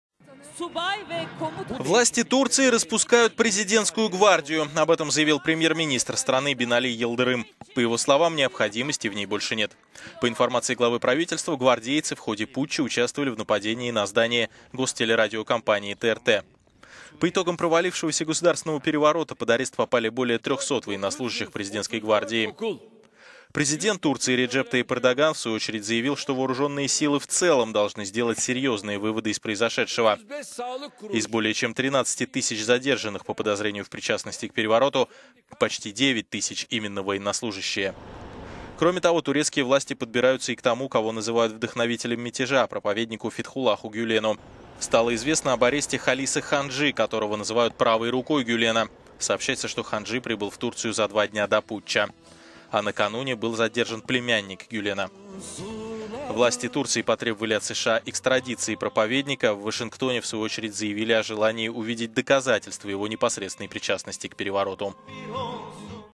Власти Турции распускают президентскую гвардию
Власти Турции распускают президентскую гвардию — об этом заявил премьер-министр страны Бинали Йылдырым. По его словам, необходимости в ней больше нет. По информации главы правительства, гвардейцы в ходе путча участвовали в нападении на здание гостелерадиокомпании TRT.

По итогам провалившегося государственного переворота, под арест попали более трёхсот военнослужащих президентской гвардии.

Президент Турции Реджеп Тайип Эрдоган в свою очередь заявил, что вооружённые силы в целом должны сделать с…